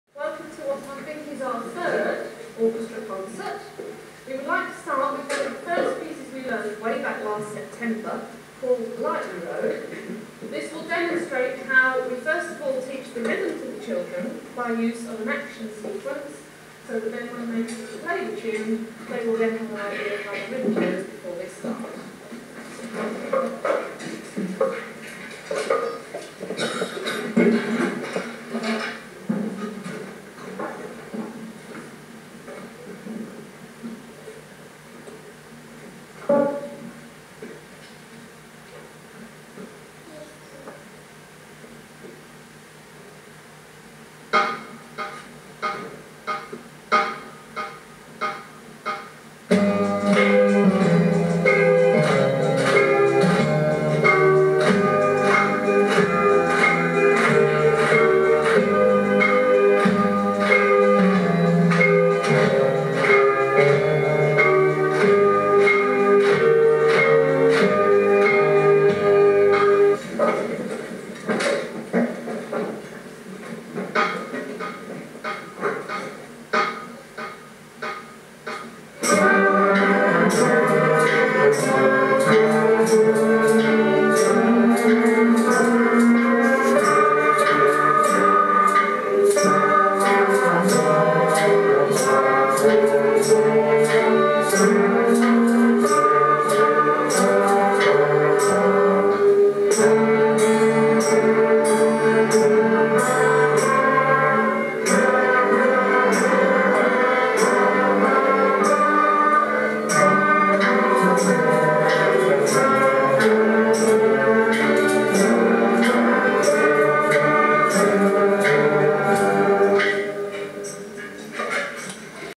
Chippenham's Kings Lodge School concert 1997
Kings Lodge School in Chippenham in 1997 and the first part of the concert given by the orchestra. Wait till they start playing!